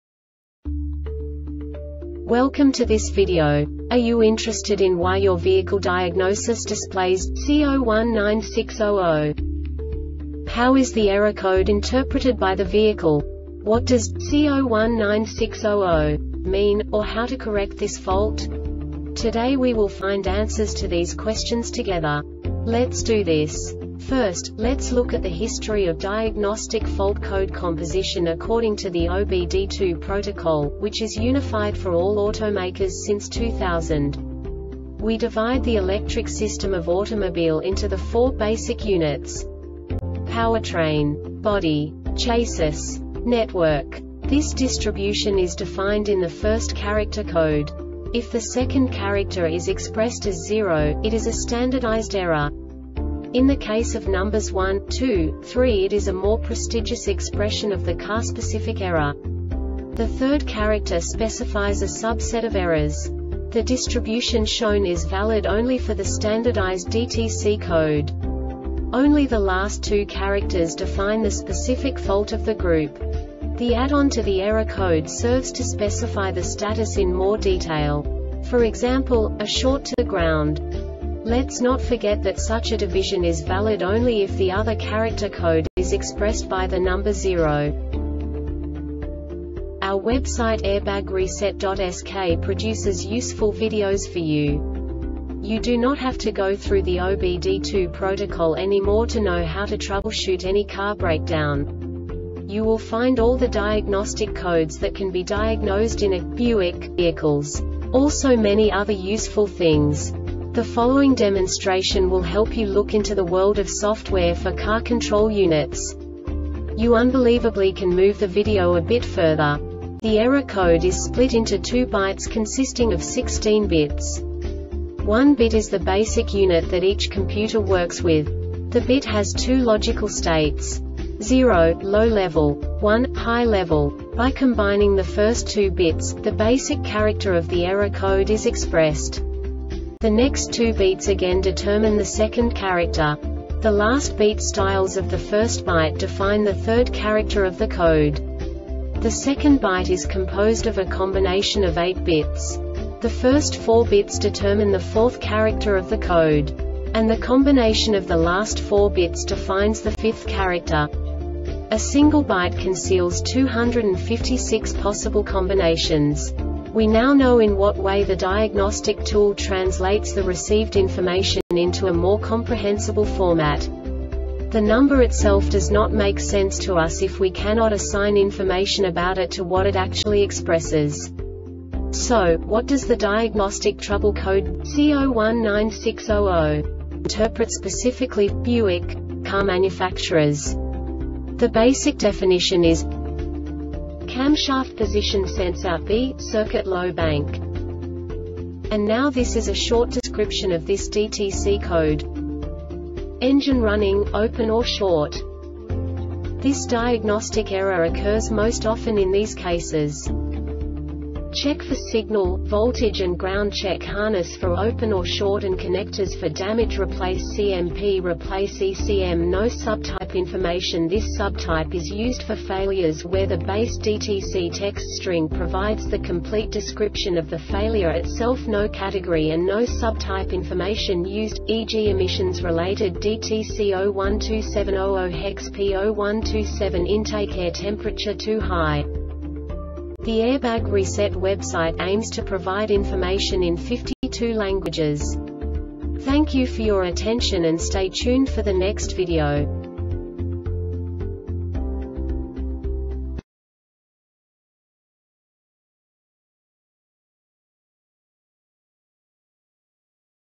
DTC Buick C0196-00 Short Explanation
Contents:
0:21 Basic DTC analysis according to OBD2 protocol standard.
1:48 Insight into programming
2:58 A diagnostically understandable description of the Diagnostic Trouble Code
3:05 Basic error definition

Make
Buick
Code
C0196 00
Definition
Camshaft Position Sensor "B" Circuit Low Bank 1
Description
Engine running,  open or short
Cause
Check for signal,  voltage and groundCheck harness for open or short and connectors for damageReplace CMPReplace ECM
Failure Type